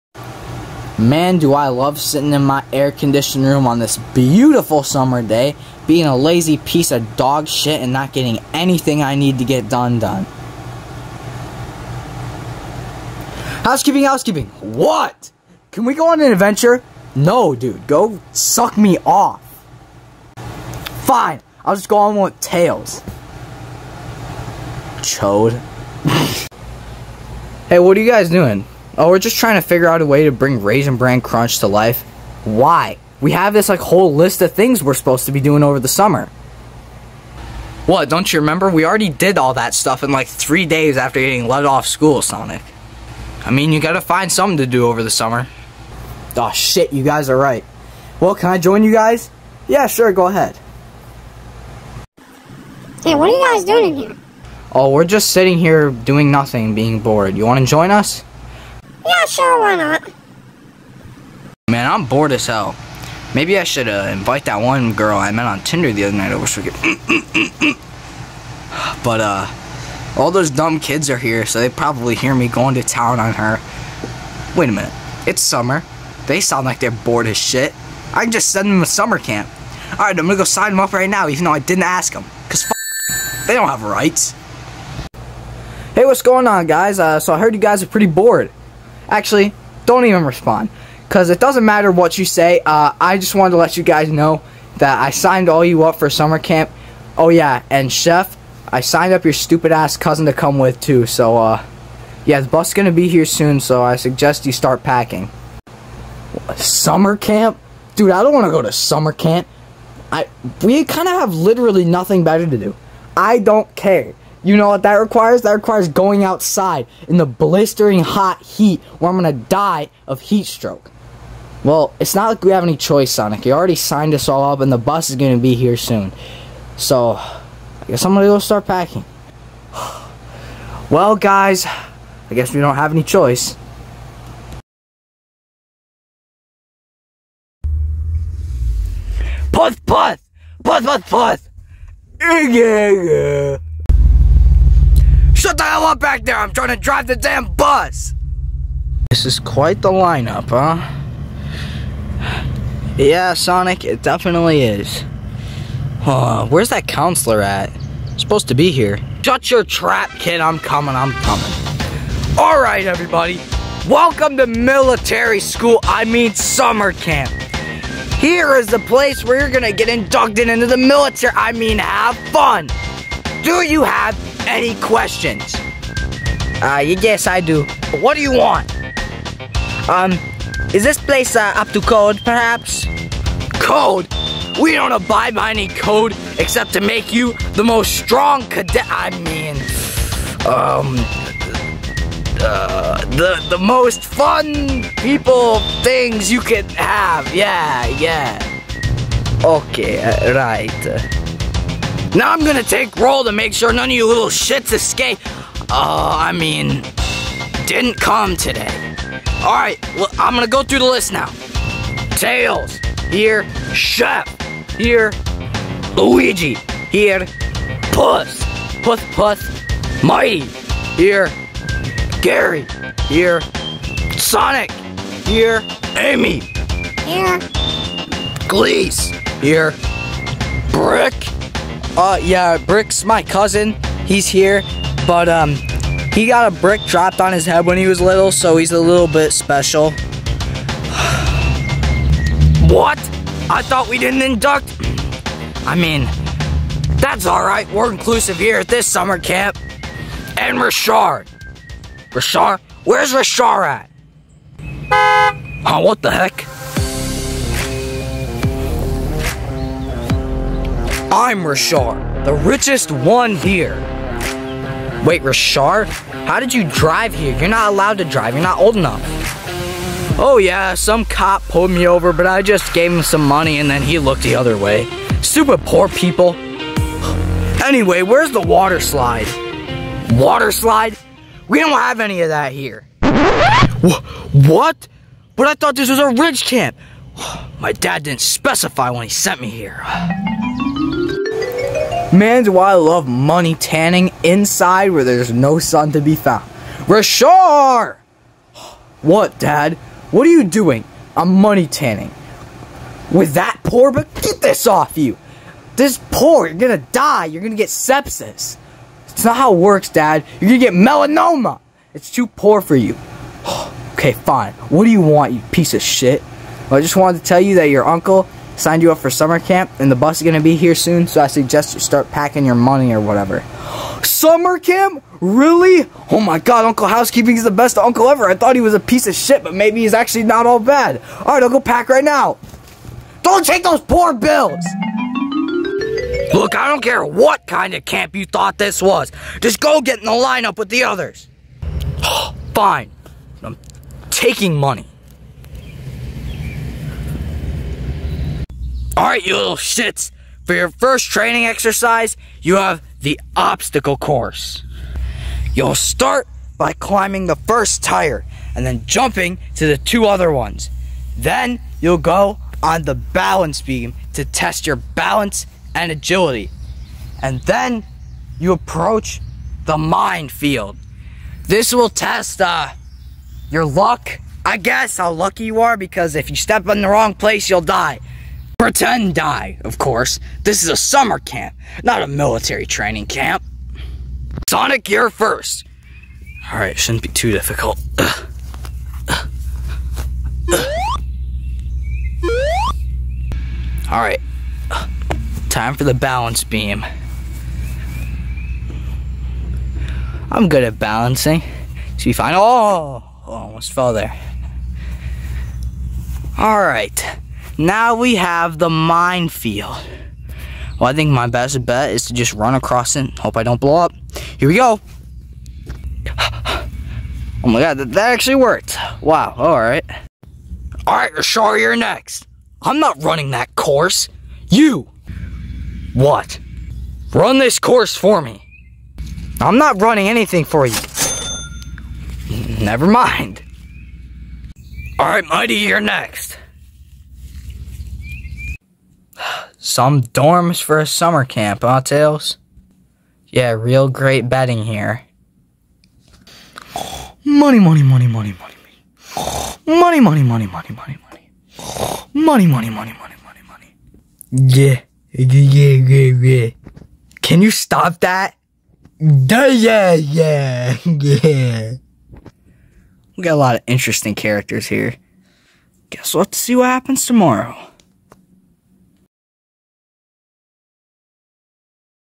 Sonic in Summer Camp REVIVED: Part 1
Brick is satire (don't cancel me)
I decided to remake this short series for fun. so don't expect the refined quality I try to have in my regular videos. I was too busy laughing at the outtakes n stuff while editing it